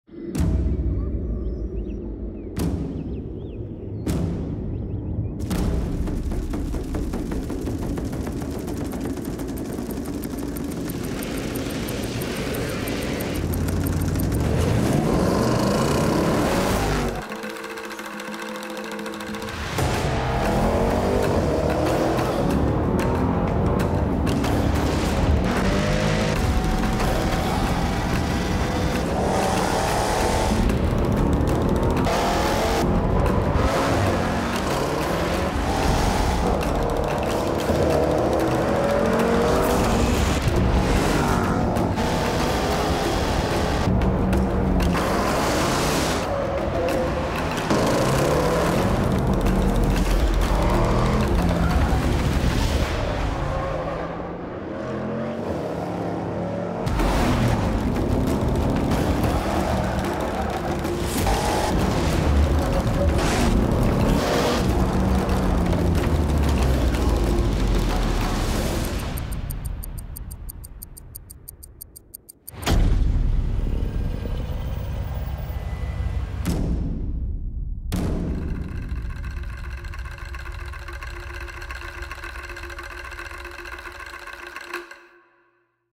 Range Rover Sport SVR | Tianmen Road Time Trial
Can a Range Rover Sport SVR beat a record set in a Ferrari?
Watch as Le Mans 24H winner Ho-Pin Tung takes our 575PS performance SUV on the hunt to set a new benchmark on the 99 turns of the Tianmen Road in China.